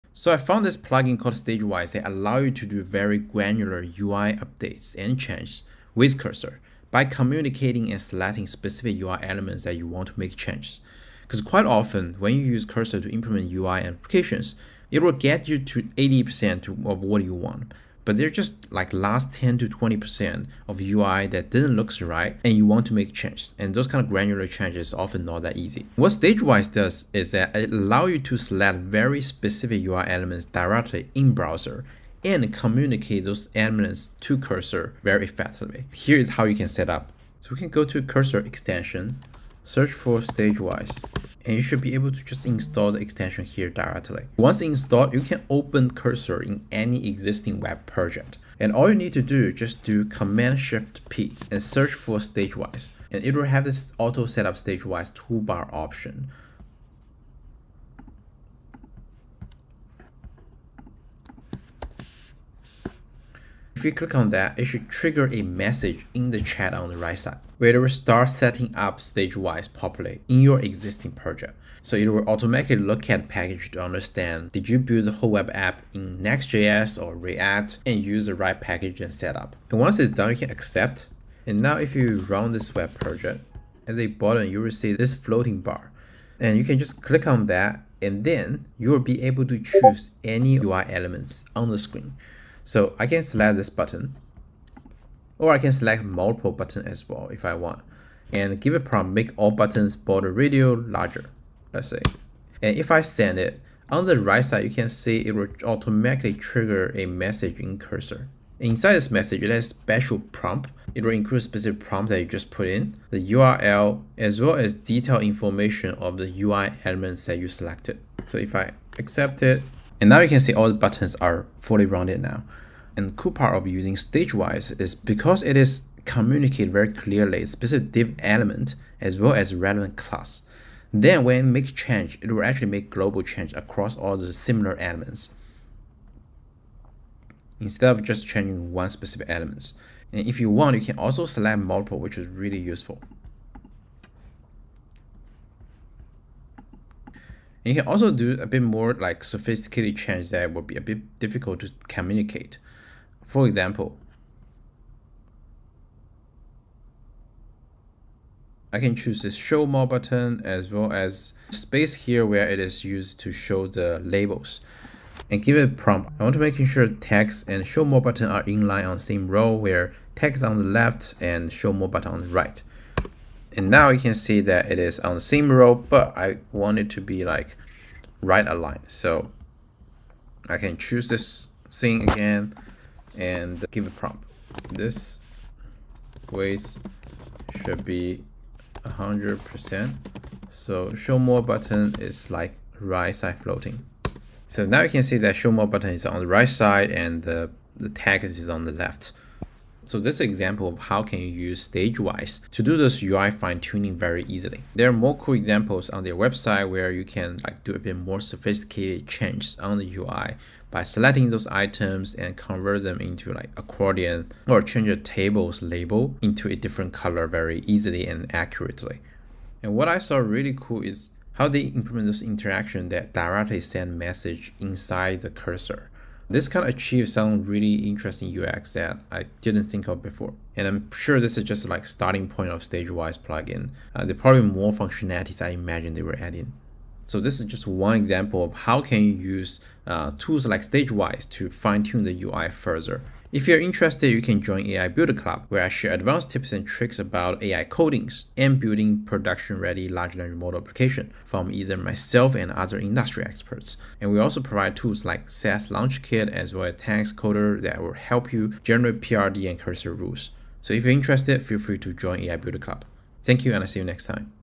How to make accurate UI Tweak in Cursor with Stagewise
⏱️ Timestamps
0:00 Intro to stagewise
0:36 Setup
1:21 Demo

👋🏻 About Me
My name is Jason Zhou, a product designer who shares interesting AI experiments & products.